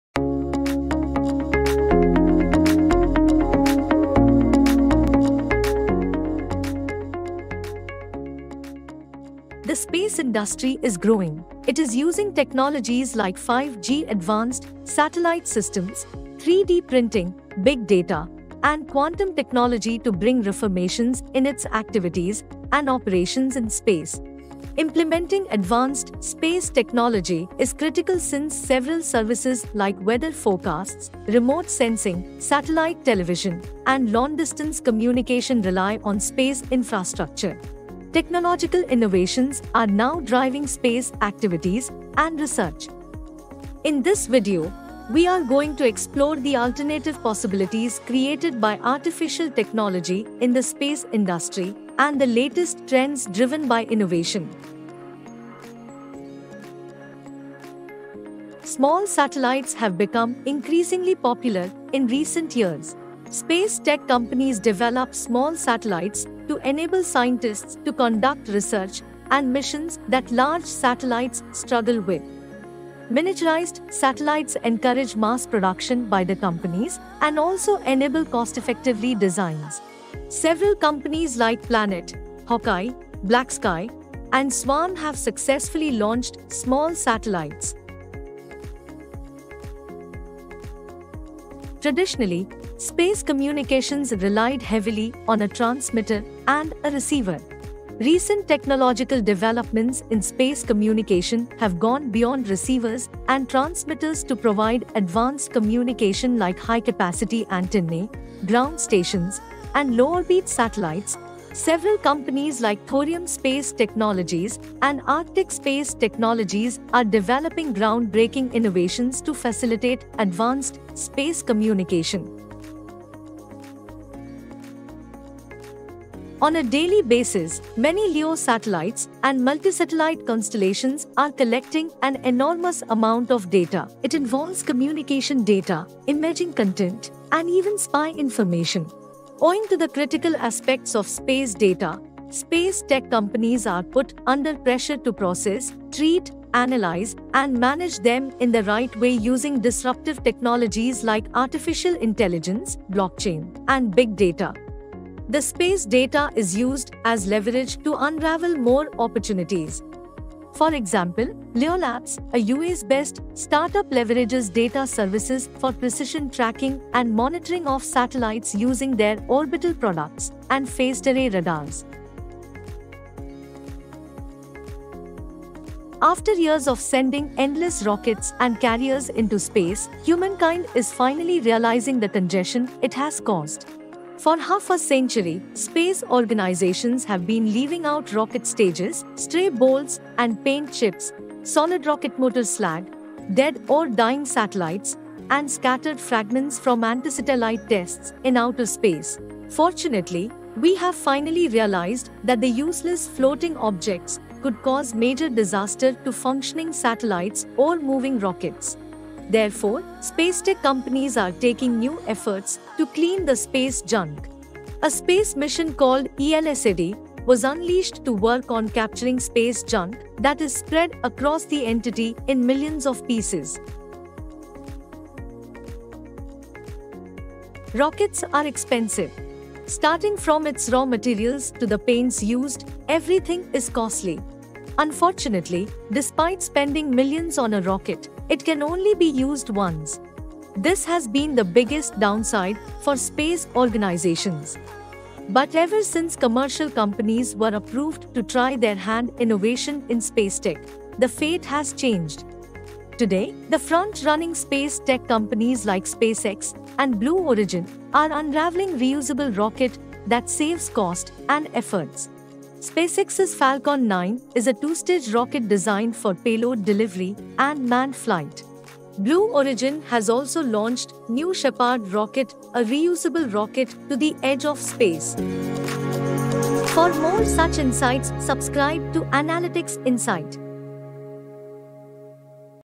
Top SpaceTech Trends to Look Out For in 2021
The space race has slipped out of government organizations. Today, tech entrepreneurs like Jeff Bezos and Elon Musk are maintaining a close race in the space tech industry by unraveling innovative solutions. Besides, disruptive technologies including 5G, advanced satellite systems, 3D printing, big data, and quantum technology are used to bring reformations in its activities and operations in space. In his video, we take you through some of the top space tech trends of 2021.